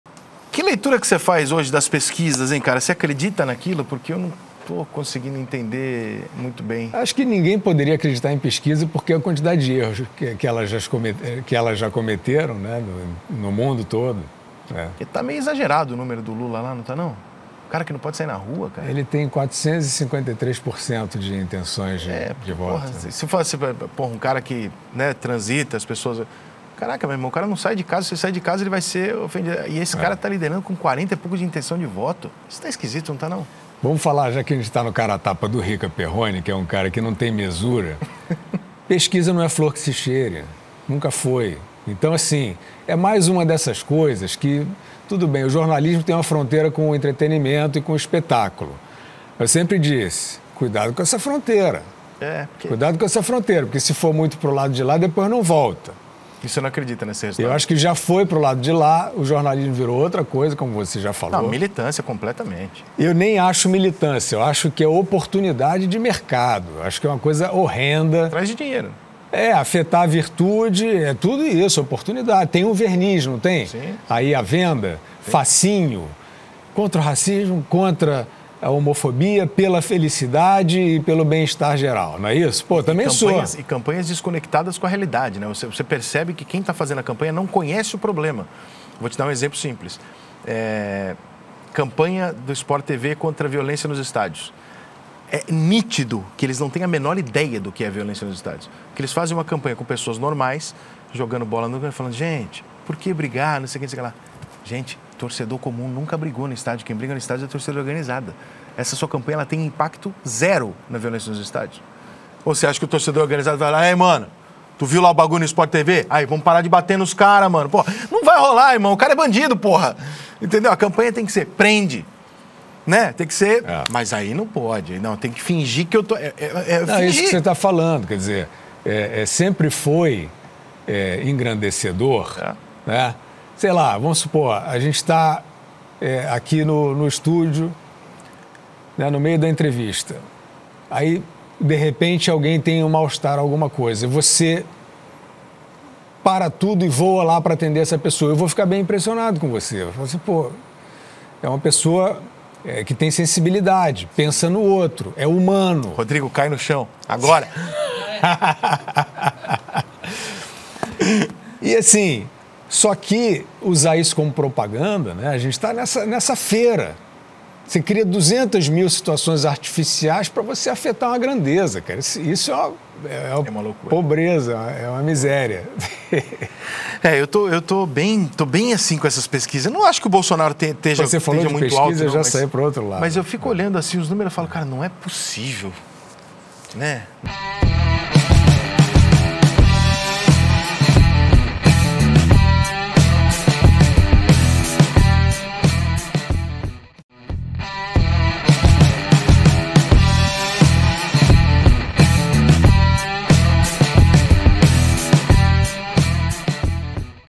VOCÊ ACREDITA NAS PESQUISAS ELEITORAIS?
Guilherme Fiuza e Rica Perrone questionam as pesquisas eleitorais, devido ao pequeno contato com a população brasileira por parte de Lula, que até pouco tempo estava preso

Produção: Estúdio Century